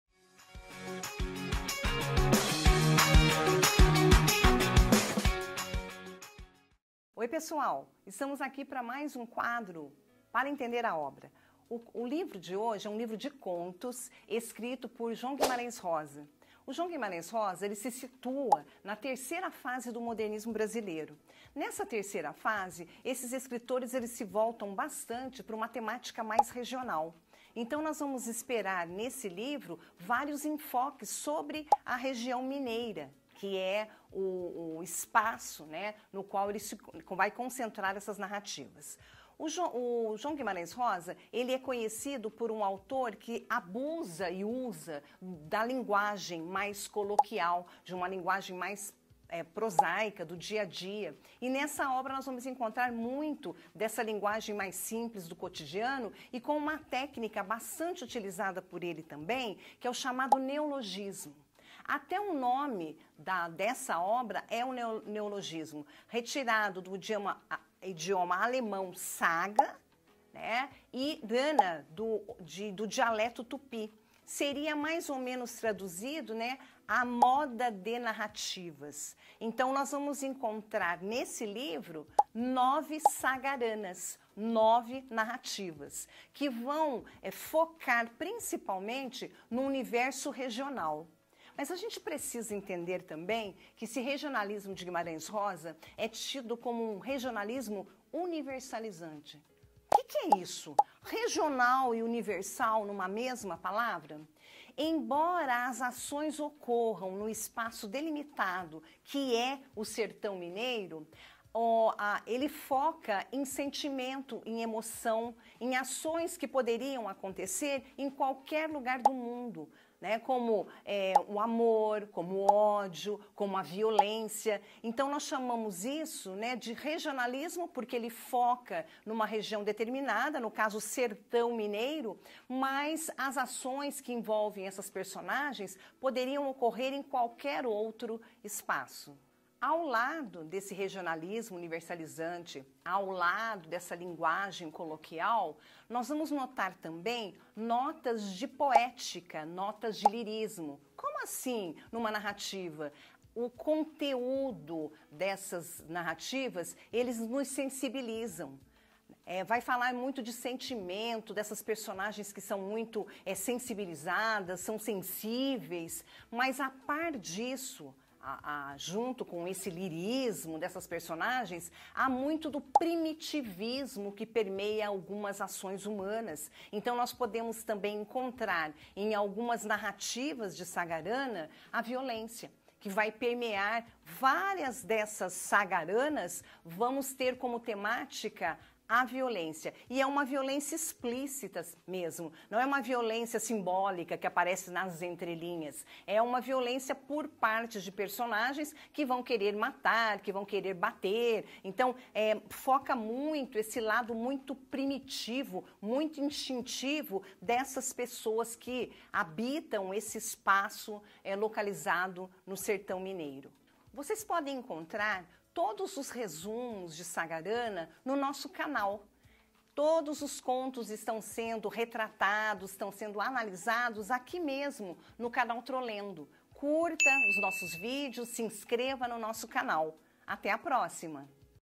Para Entender | Sagarana | Vestibular Unicentro 2024
Uma coletânea que reúne 9 contos ambientados no sertão mineiro e que, ao retratar o universo dos sertanejos, trata de temáticas universais com uma linguagem coloquial. Essa obra é Sagarana, de Guimarães Rosa.

Escritor da Terceira Fase do Modernismo, Guimarães Rosa utiliza uma linguagem inovadora, repleta de regionalismos e neologismos, criando um estilo próprio que mescla o popular com o erudito. As narrativas de Sagarana exploram temas como a honra, o destino, o amor, a morte e a vingança, sempre dentro de um contexto em que o sertão é quase um personagem por si só.

Nesse vídeo você pode conferir os principais aspectos do contexto da obra, que está listada para o vestibular de 2024 da Unicentro.